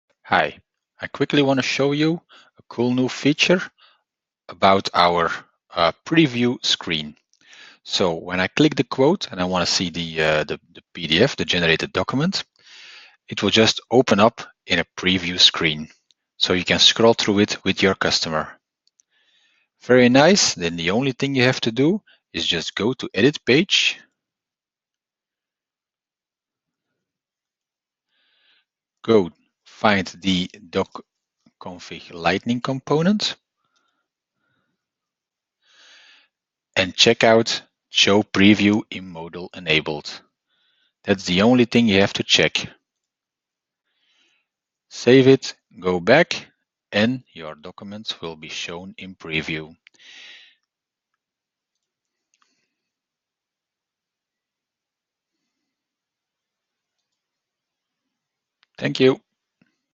Summer 19 Preview mode
This video shows how a cool preview can be shown easily. Just configuration of the Lightning component